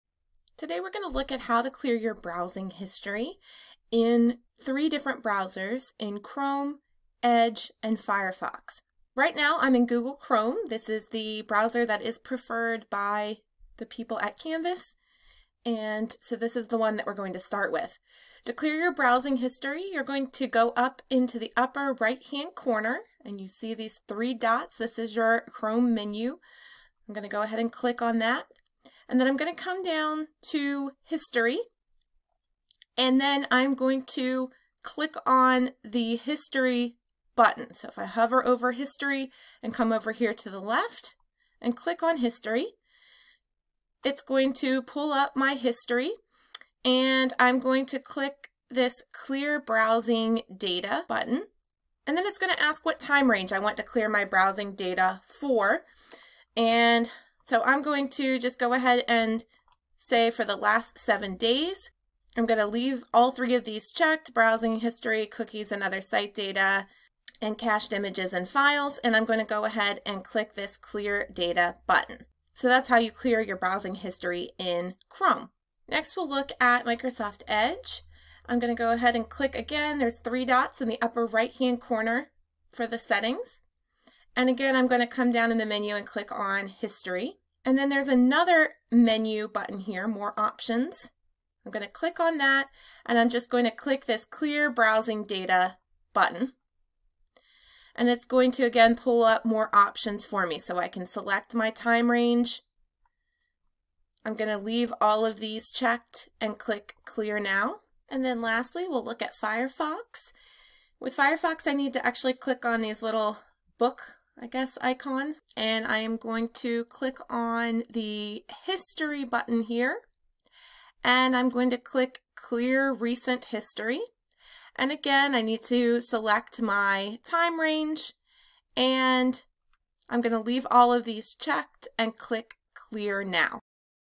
How to Clear your Browser History
This video will show you how to clear your browsing history in Chrome, Edge, and Firefox.